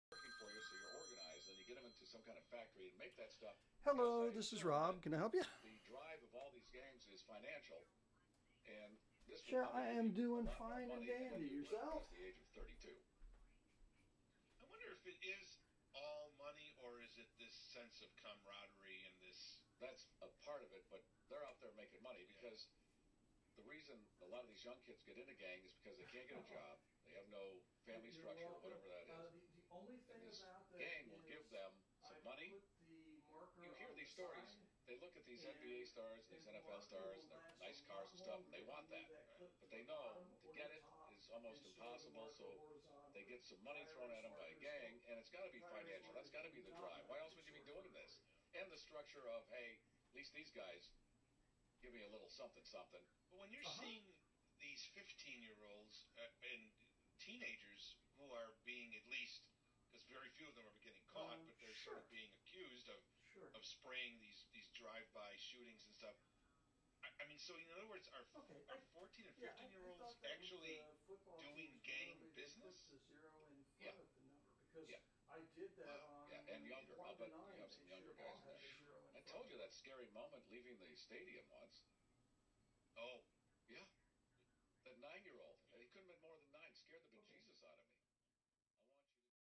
How I Listen to Garry Meier, WGN.FM
I have a home-based business.  Here’s how I listen to the Garry Meier show since he’s been moved from the AM dial to a streaming format on the Internet.